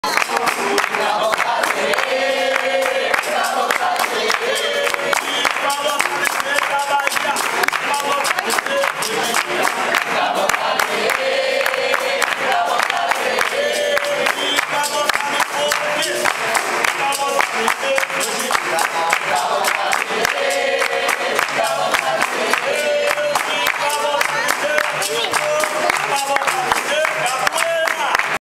XII Batizado Porto da Barra Cadiz
16 y 17 Mayo 2010 (Chiclana)
Organiza: Alumno Avanzado Olodum
Supervisiona: Mestre Pititinga